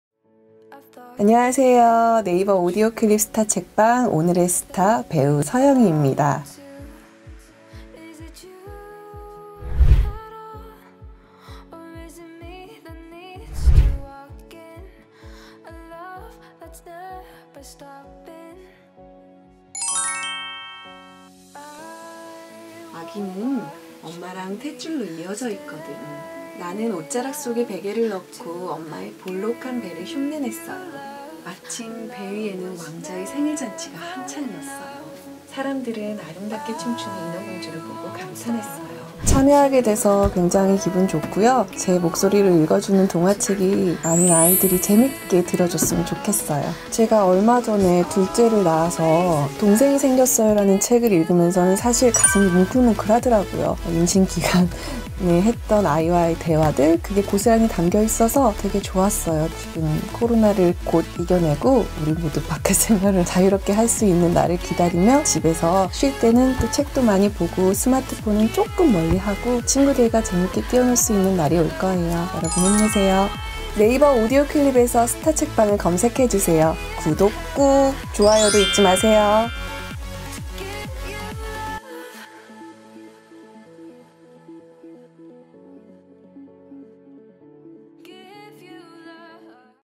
[텐아시아 스타책방] 릴레이 재능기부 - 서영희 편 "동화책 읽는 내내 가슴이 뭉클했어요"
배우 서영희가 텐아시아와 네이버 오디오 클립이 주최한 '스타책방'에 일곱 번째 주자로 참여했다. '스타책방'은 스타들의 릴레이 재능기부 프로젝트로 청각장애 부모를 둔 아이들, 다문화 가정의 아이들에게 동화책을 읽어주는 취지로 제작됐다.

서영희는 그레이트북스 내 친구 과학공룡의 '동생이 생겼어요'와 그레이트북스 행복한 명작의 '인어공주'를 읽었다. 그는 따뜻한 목소리로 몰입도 높은 리딩을 펼쳤다. 이어 훈훈한 감성으로 모성애를 자극했다.

녹음을 마친 후 서영희는 "참여하게 돼서 굉장히 기분이 좋다"면서 "내 목소리로 읽는 동화책을 많은 아이가 재밌게 들어줬으면 좋겠다"고 밝혔다.

지난 5월 2일 둘째를 득녀한 서영희. 그는 "'동생이 생겼어요'라는 책을 읽으면서 가슴이 뭉클했다"며 "임신 기간에 했던 아이와의 대화들이 고스란히 담겨 있어 정말 좋았다"고 털어놓았다.

그러면서 "코로나를 이겨내고 자유롭게 활동할 수 있는 날이 올 것"이라고 전했다.

'스타책방'은 매주 월, 목요일 네이버 오디오 클립을 통해 공개되며 네이버 부모i 판에서도 만날 수 있다.